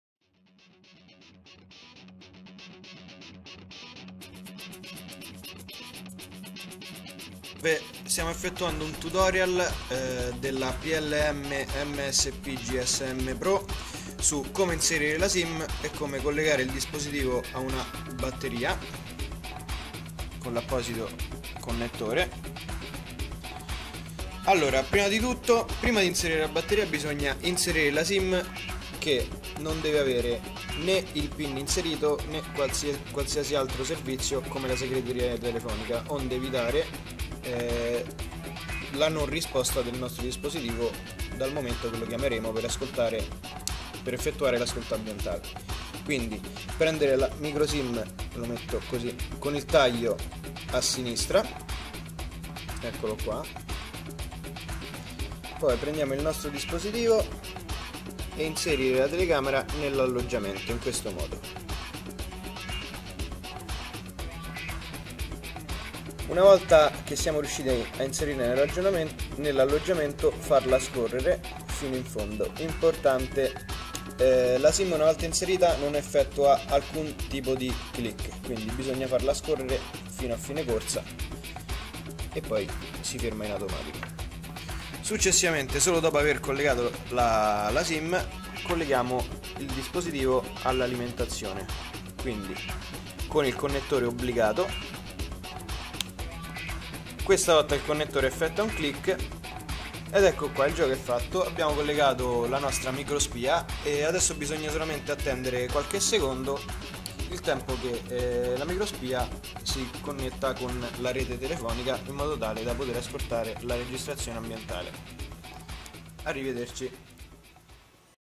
Microspia per Auto Cimice GSM a lunga durata
Video Tutorial sull'utilizzo della microspia gsm per auto